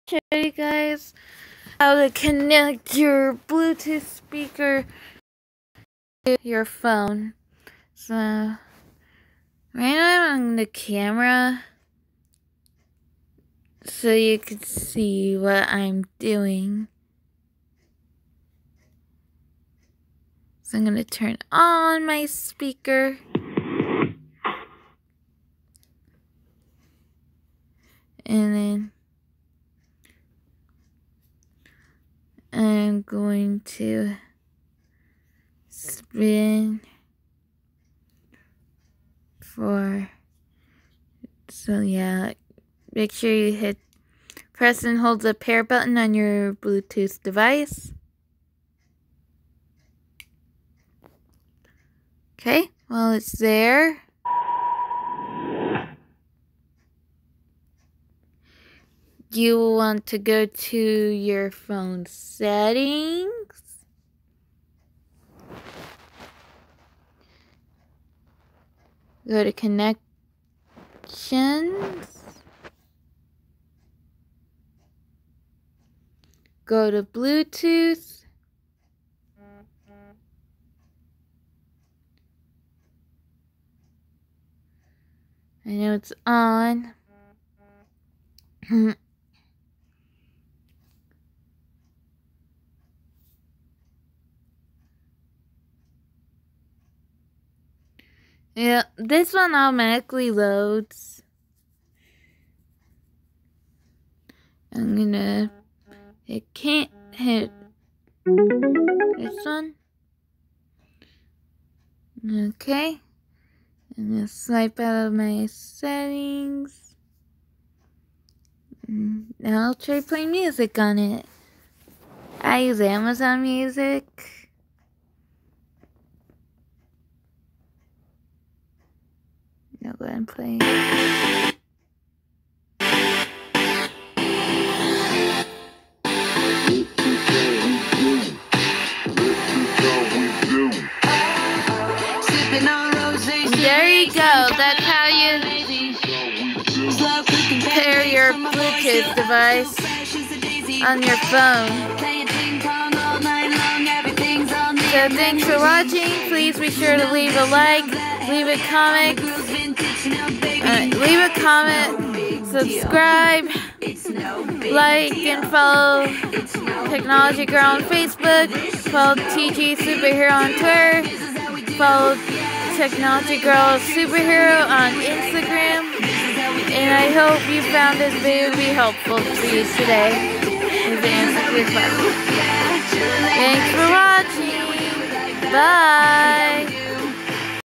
How to pair a Bluetooth device to your mobile device
This video shows you how to pair a bluetooth device to your phone or any mobile device. I hope this video helps you today